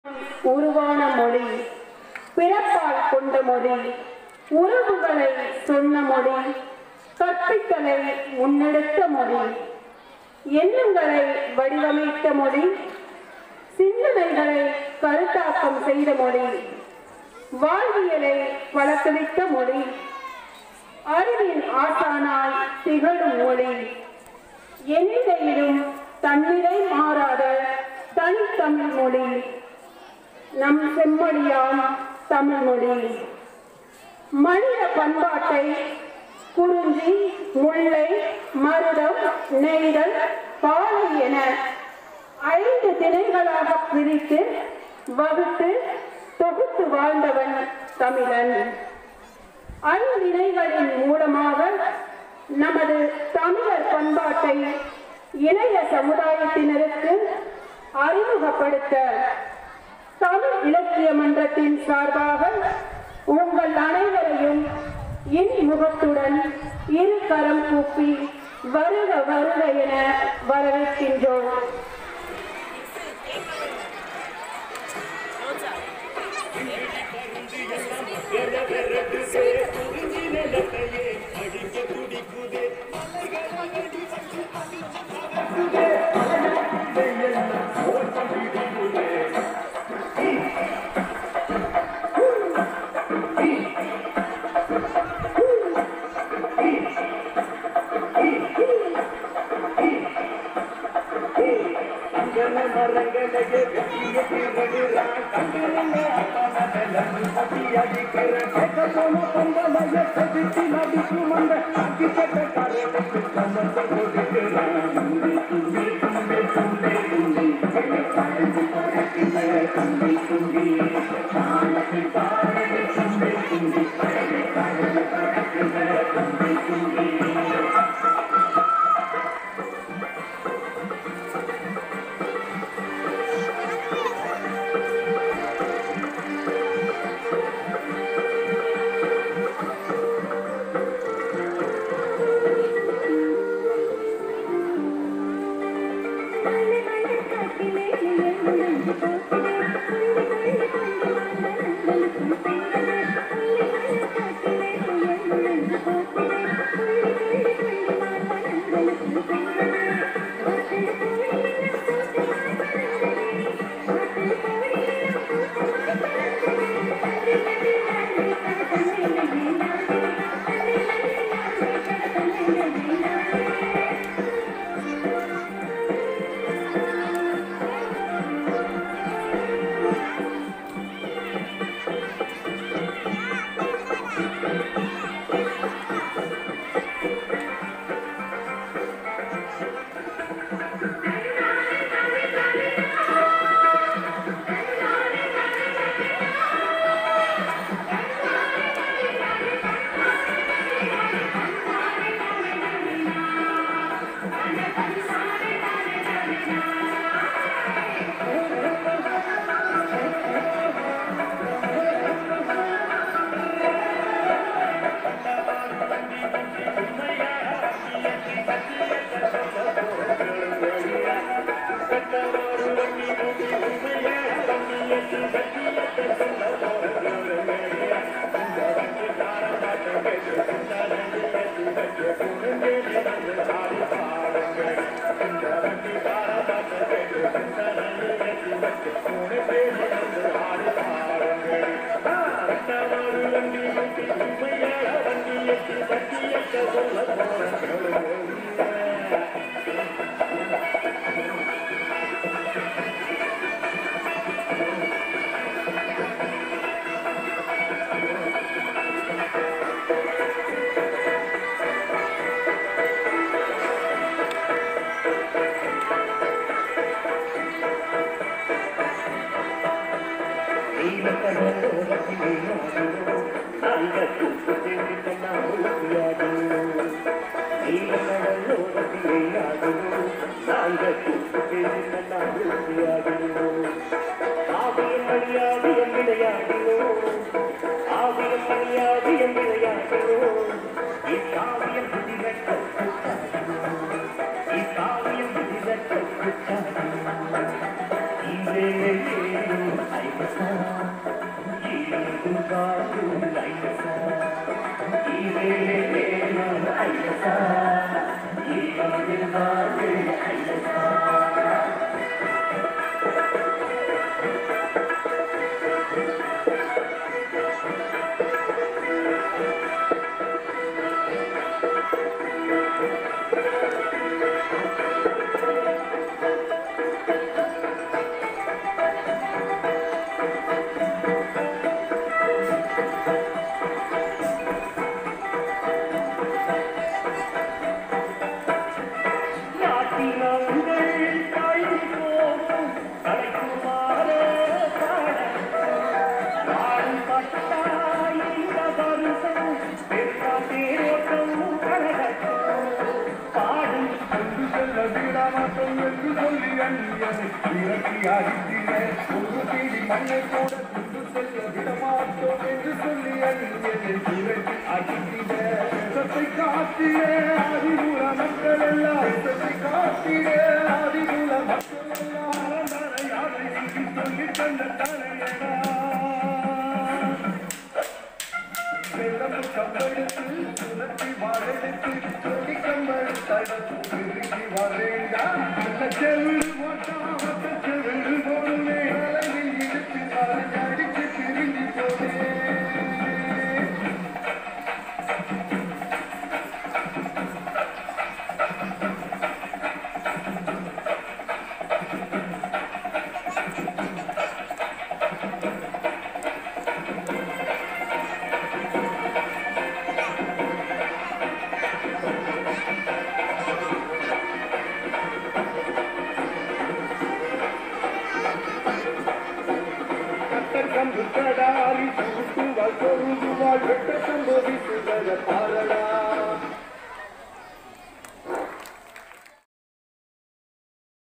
MARY IMMACULATE - TAMIL CLUB 2023
TAMIL CLUB INAUGURATION STUDENTS PERFORMED WELL... VIDEO PREPARED BY MS.SHRINITHI.